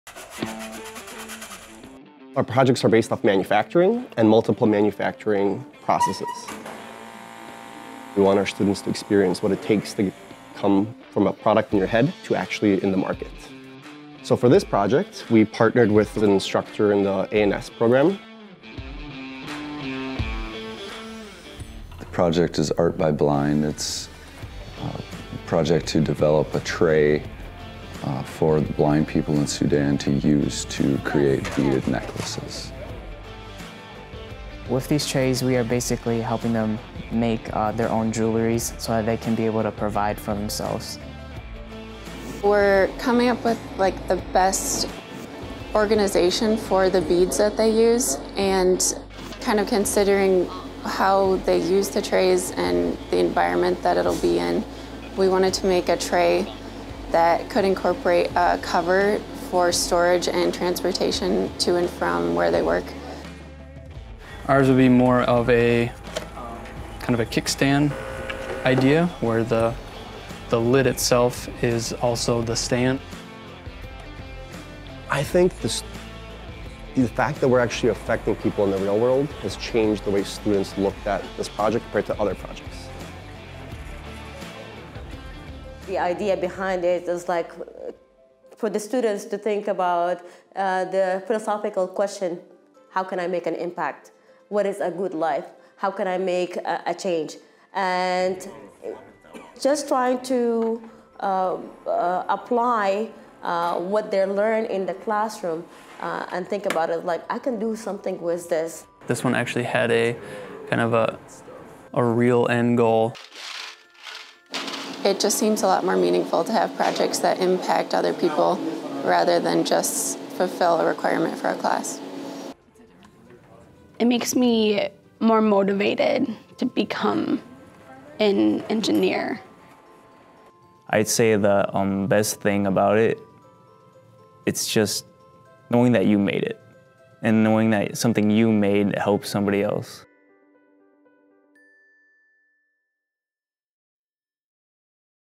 Dunwoody students design trays for blind Sudanese bead artists
Dunwoody students in the Engineering Drafting & Design program use their skills to help transform the lives of blind bead artists in Sudan.